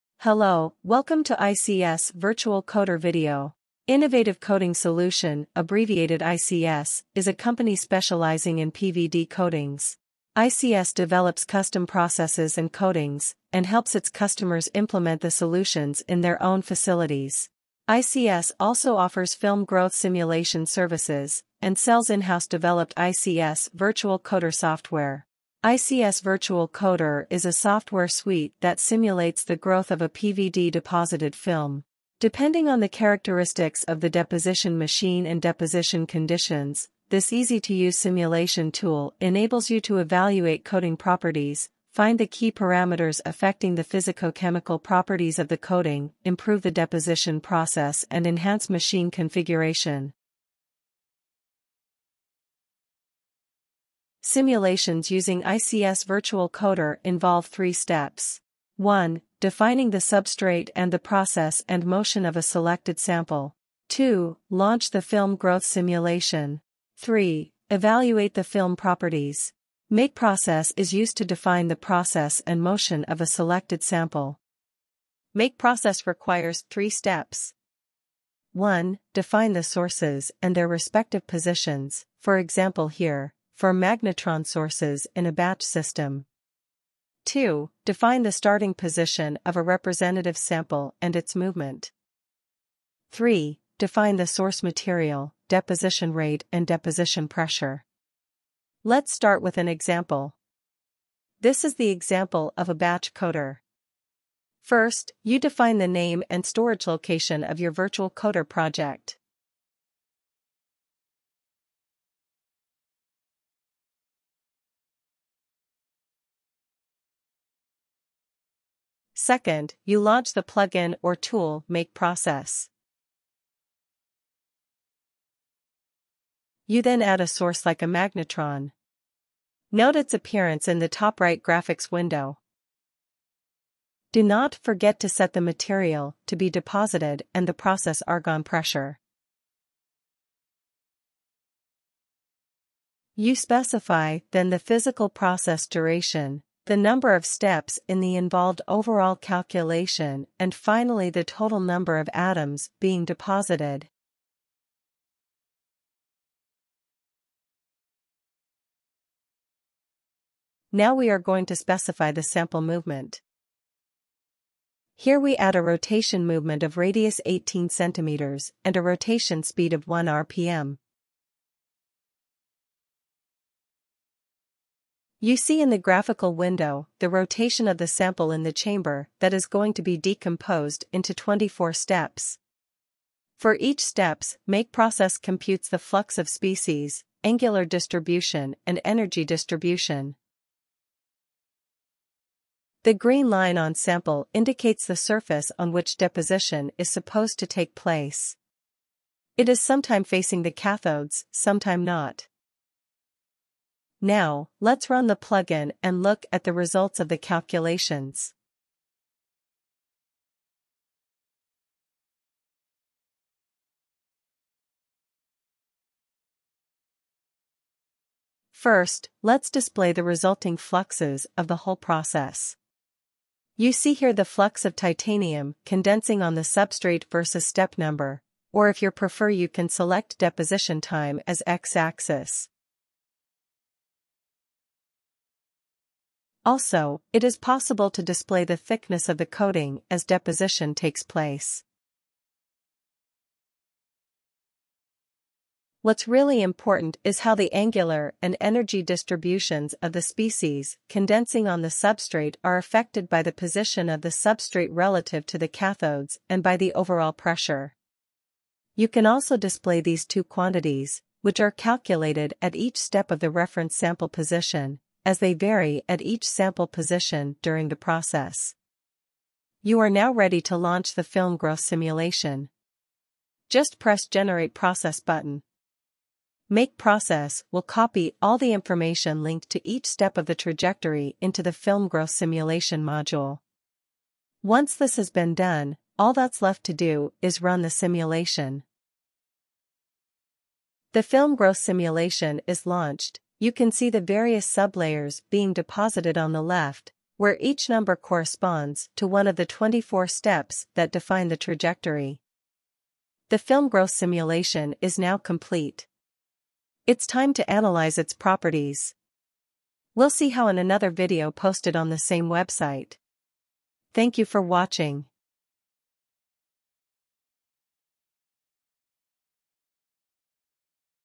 make process 1 batch coater
ICS Virtual PVD coater: makes use of the make process plugin to setup deposition in a batch deposition machine equipped with 1 cathode and a one-fold rotating sample.